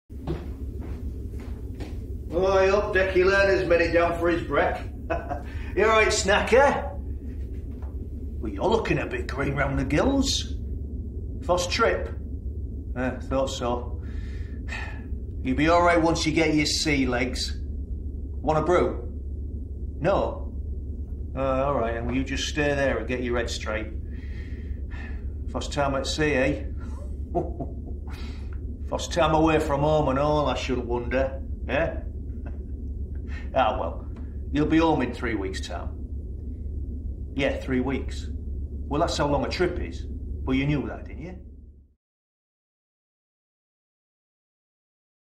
Tales from a Trawlerman - Trailer
If you are covering the history of Hull’s fishing industry with your class, then why not watch our Tales from a Trawlerman video series?

In this set of five videos, your pupils will join a trawlerman on board the Arctic Corsair and get an inside look at life as a fisherman. The videos are also supported by some helpful resources to use with your class.